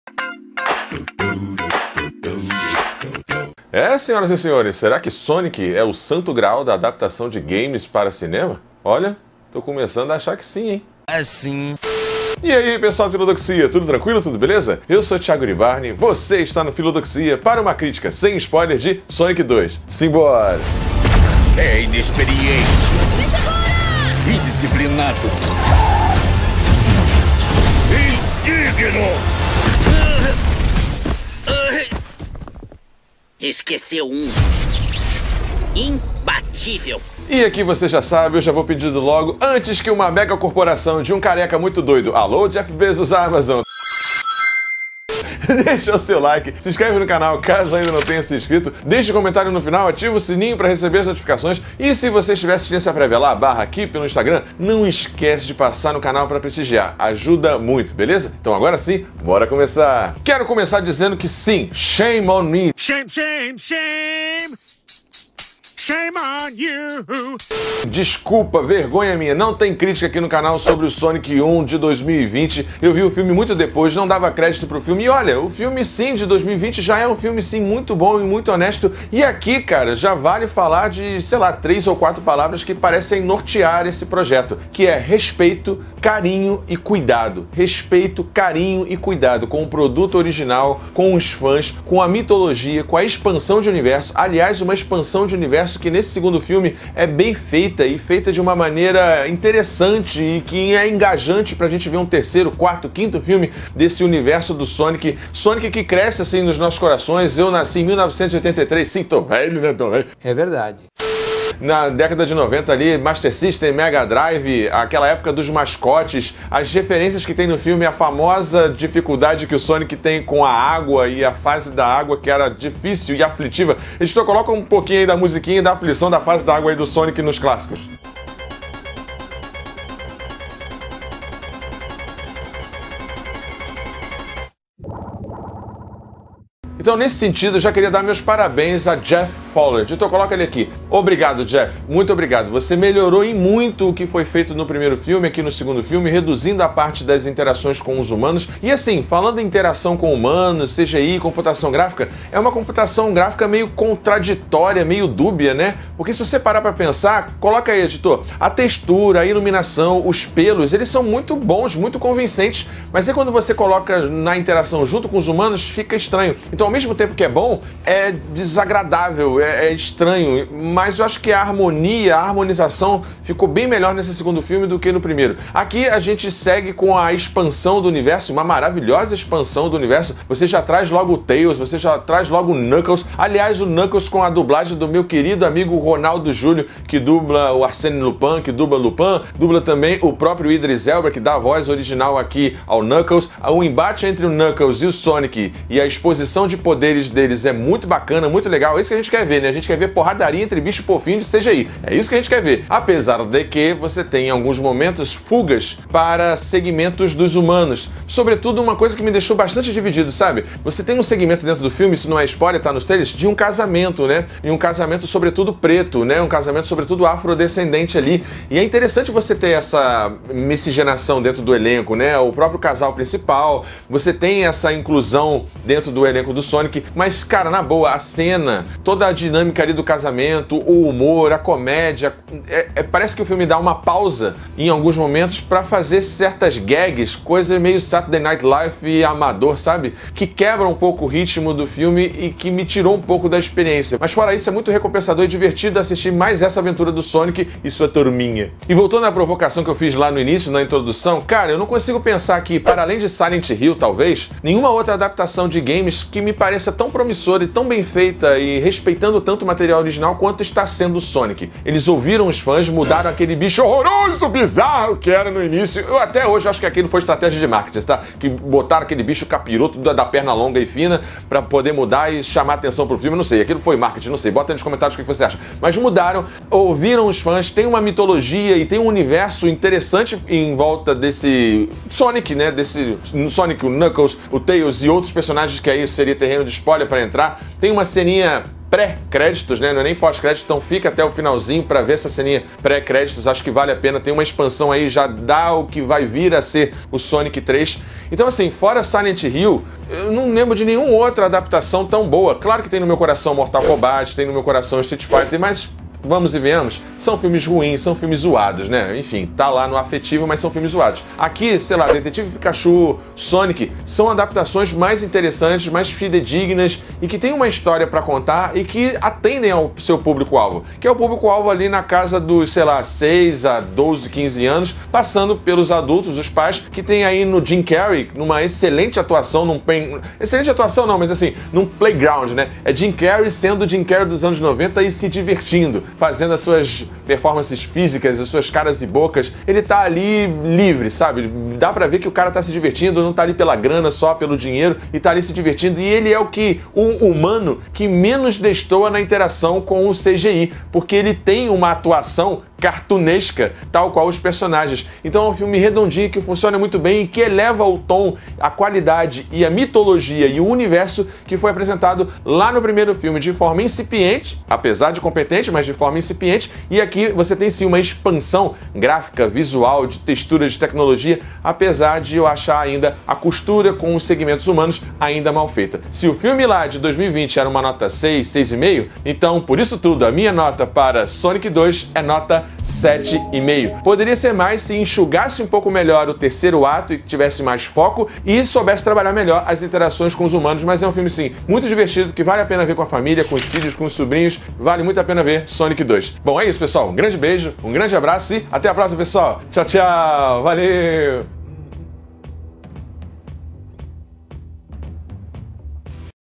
SONIC 2 - O PODER DO AZULZINHO! 🤪
Adaptação definitiva de games chegou? A interação com humanos ainda atrapalha? É melhor que o primeiro de 2020? Isso e mais nessa análise SEM SPOILER. Gostou do vídeo? Curte, comenta e compartilhe.

✨Roteiro e apresentação: @filodoxia_prof.iribarne
Edição: @talesbarradas

✂️ ↔️ Capítulos / Minutagem

0:00 Introdução
1:05 Uma adaptação de respeito
2:31 Esse CGI...
2:58 Multiverso Sonic e dublagem BR
3:26 Esses humanos...
4:11 O filme definitivo de games!
5:29 O Jim Carrey que amamos
5:57 Nota e considerações finais